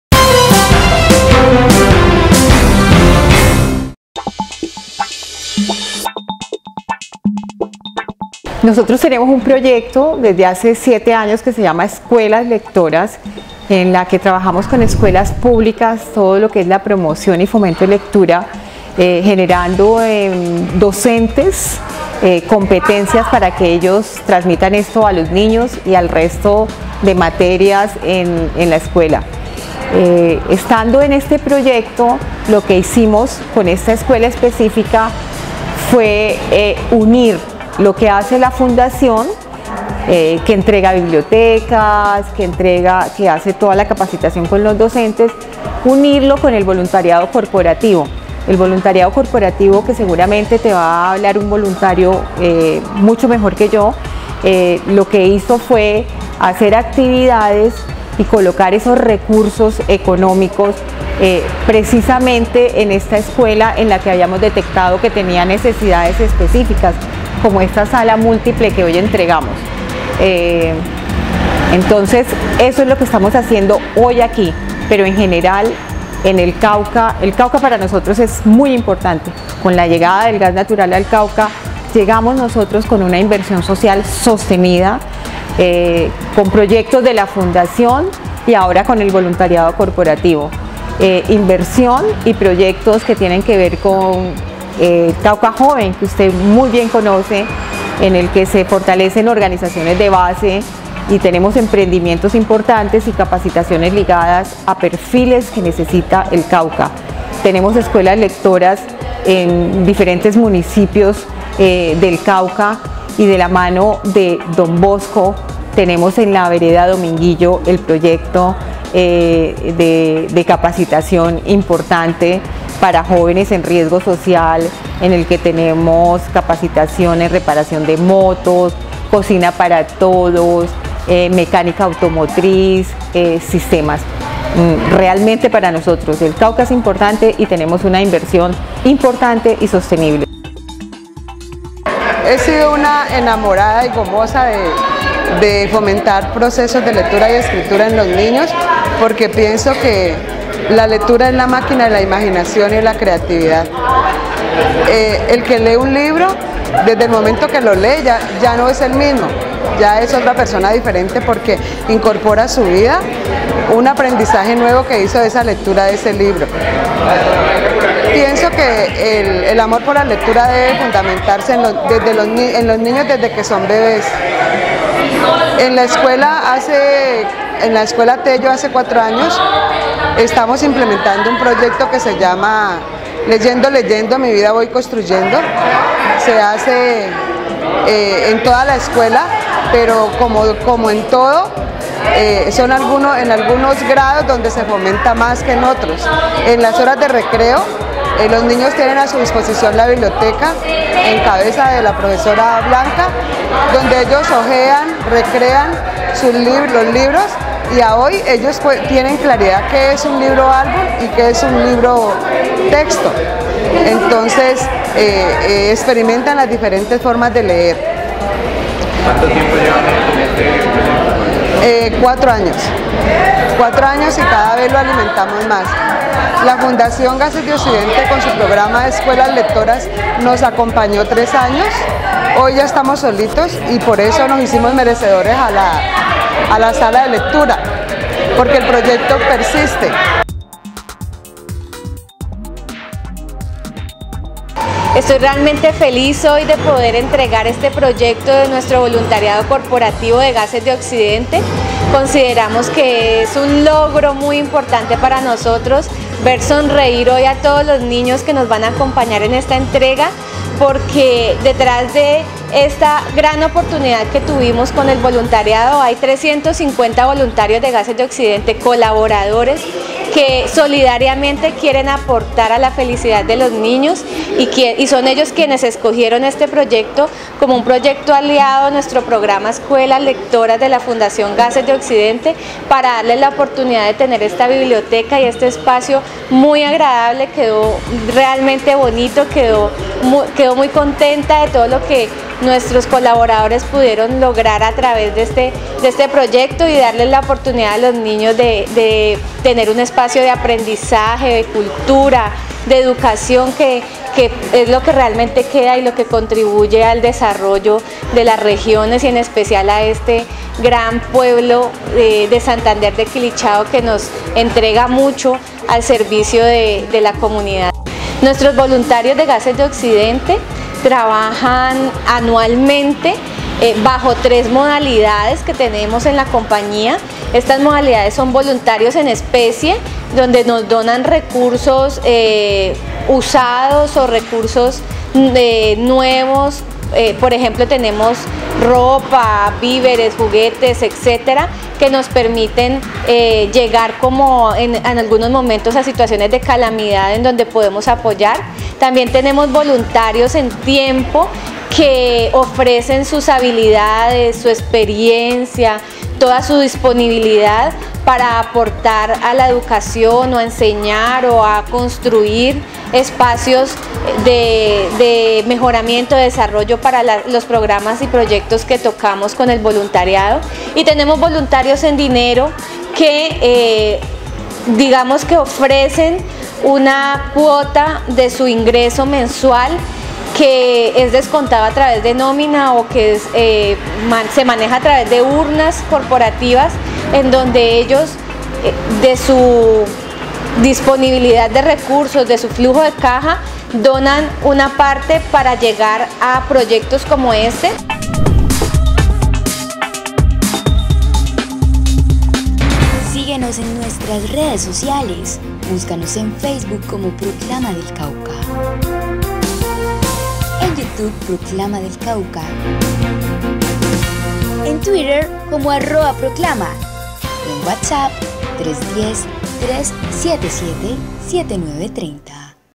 Escuelas Lectoras en el Cauca apoyadas por la Fundación Gases de Occidente
Niñez de Santander de Quilichao se beneficia del proyecto “Escuelas Lectoras”

El viernes 5 de diciembre de 2014, un hecho de alegría colmó a los niños de la Escuela Pública Rafael Tello, con la donación de una biblioteca que, a través de la lectura, la Fundación Gases de Occidente y el Voluntariado Corporativo “Granito de Arena” beneficiaron a más de 600 niños de Santander de Quilichao, Cauca, con el proyecto “Escuelas Lectoras” que desde hace 7 años busca formar, asesorar y acompañar a los docentes en el uso de herramientas y estrategias que fortalecen los procesos de promoción y comprensión lectora de estudiantes de básica primaria.

“La Fundación Gases de Occidente siempre está presta para hacer tejido social, sobre todo en la región del Cauca, con proyectos como “Cauca Joven”, iniciativa que busca mejorar los indicadores de empleabilidad y emprendimiento de más de 200 jóvenes de los municipios de Guachené, Puerto Tejada, Villa Rica y Padilla, quienes además, podrán fortalecer iniciativas para su desarrollo socieconómico individual y colectivo; otro proyecto es el de la “Alianza Centro de Capacitación Don Bosco” donde se busca técnica y tecnológicamente actualizar los talleres de Culinaria, Mecánica Automotriz, Mecánica de Motos y Electricidad para también capacitar a la comunidad del Norte del Cauca en “Cocina para Todos”, panadería y pastelería, mecánica automotriz y de motos, sistemas, salud y belleza. Y ahora con el proyecto “Escuelas Lectoras” con el objetivo de reforzar la educación de los niños para que tengan una mejor calidad de vida y estén mejor preparados en el futuro cuando tengan la posibilidad de acceder a cupos en la educación superior” explicó a Proclama del Cauca, Melba Pinedo, directora de la Fundación Gases de Occidente.

“La lectura no la tenemos como hábito, y creo que a buena hora llegó este momento para que los niños y niñas de la Escuela Rafael Tello, desde las aulas aprendan a acercarse a los libros sin tener miedo”, afirmó la profesora guía de este proyecto en Santander de Quilichao, Blanca Disney de Tafurth.